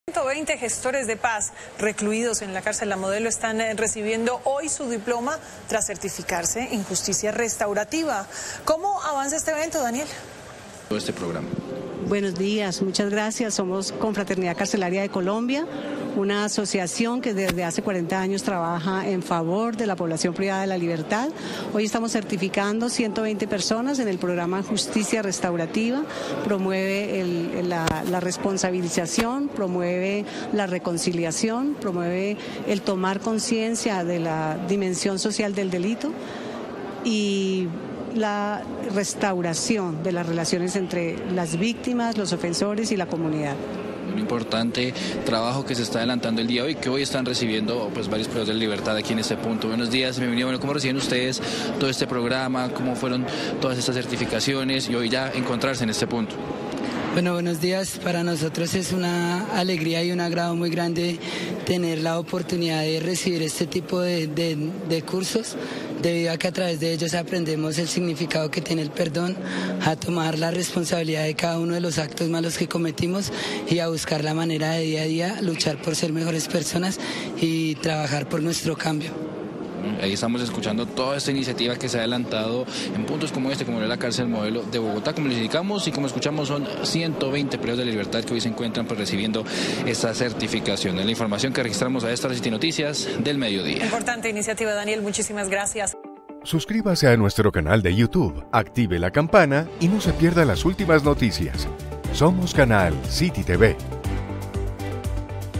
Certifican a 120 gestores de paz en La Modelo bajo programa de justicia restaurativa | CityTv
Un grupo de 120 reclusos de la cárcel La Modelo de Bogotá recibió su certificación en justicia restaurativa. El evento, organizado por la Confraternidad Carcelaria de Colombia, destacó el esfuerzo por promover la responsabilización, reconciliación y conciencia social entre los participantes.